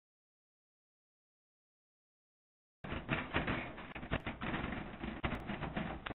the shaky cultist
some toying with filters and cuts to create weird movements in the final video